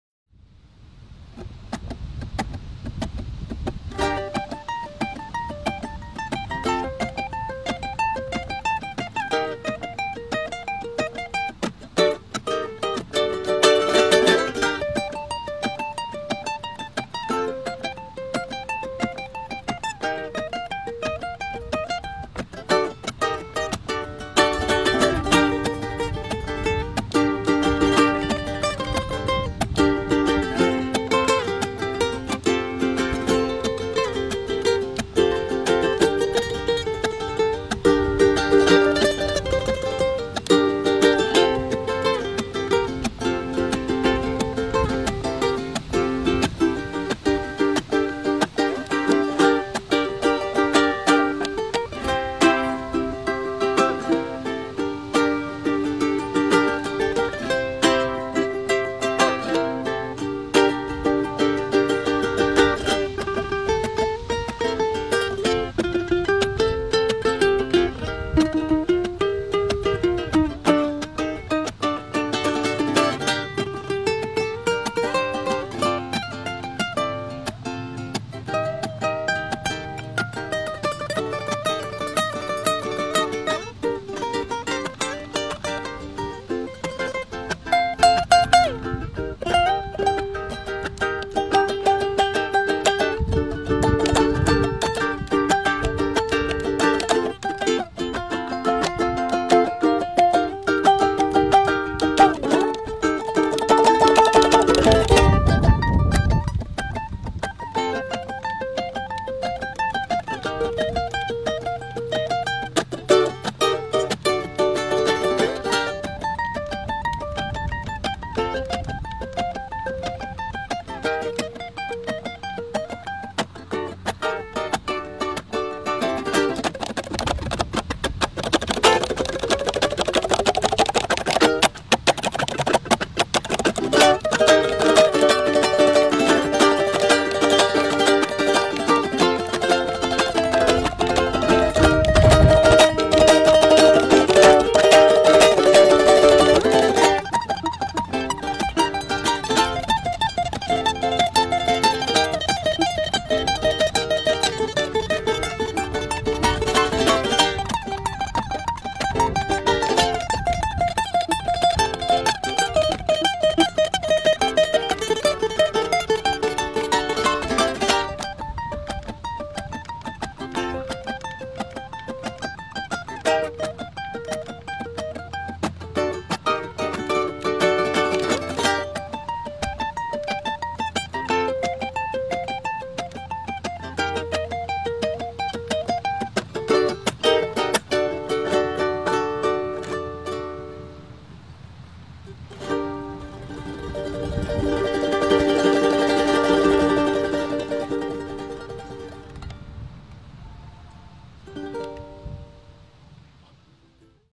Awesome Guitar Play by these two girls!!!
Enjoy this awesome Guitar Play by this two girls.
What a superb music...